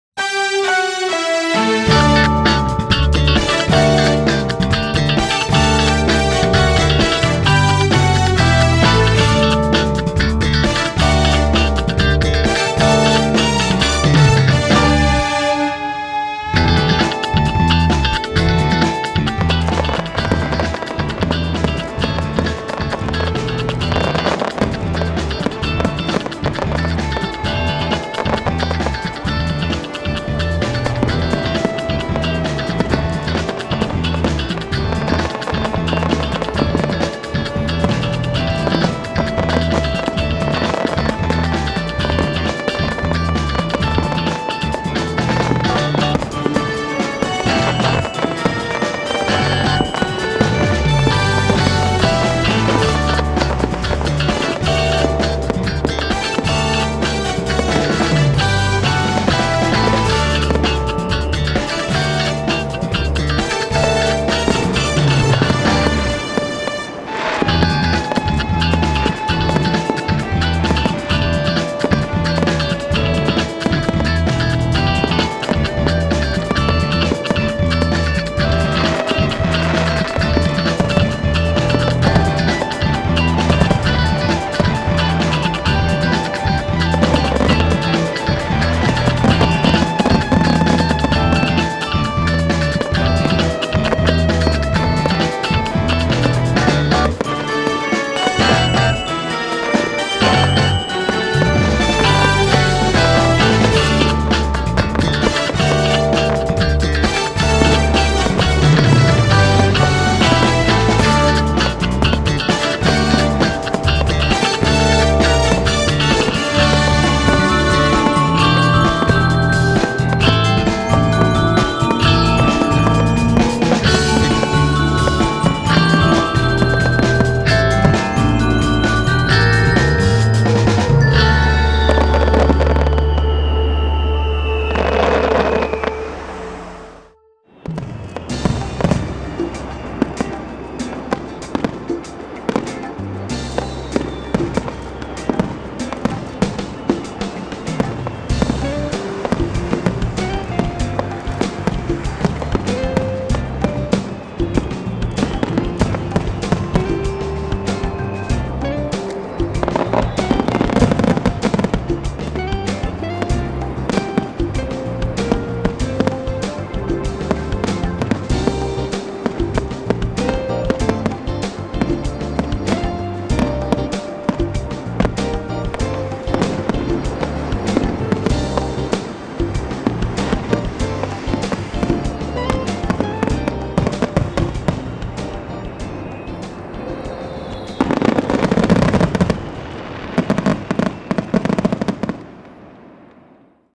Japan's fireworks／花火大会
15日の夜に打ち上げられた花火。
天候もよくとても楽しみにしていましたが・・・
なんと無風で、花火中盤あたりから煙で花火自体の
きれいな光がみれませんでした。
毎年とても楽しみにしていたのですが、今年はちょっと
いまいちでした(ーー;)